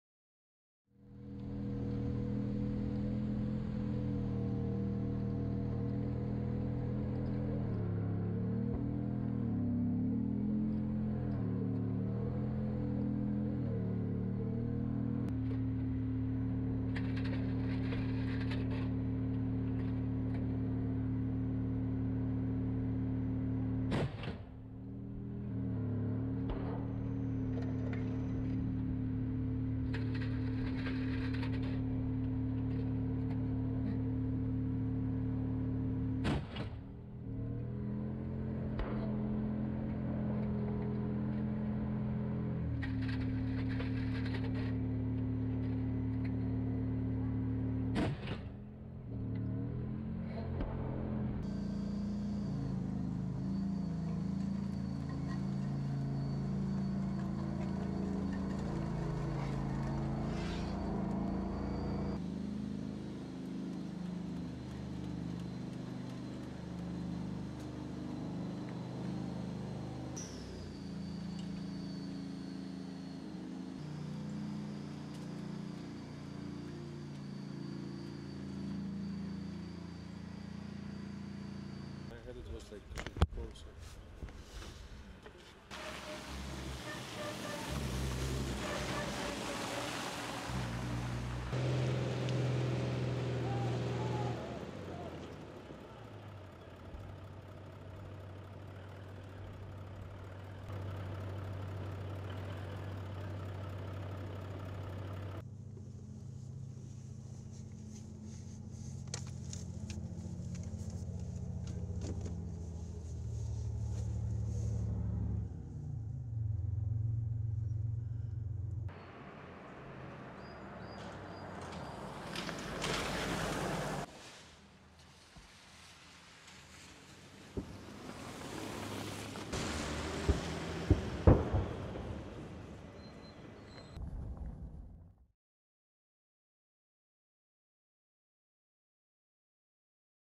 Ukrainian Army fired on Russian positions somewhere near Bakhmut to support theirs infantry
Ukrainian tanks fire shells near Bakhmut amid bloodiest battle of war, Ukraine Russia Military Video, GoPro Cam Footage

Ukrainian Army fired on Russian positions somewhere near Bakhmut to support theirs infantry. Ukrainian Army destroyed Russian infantry dogouts, as a main task. They hope to receive new military vehicles, especially Abrams and other modern tanks.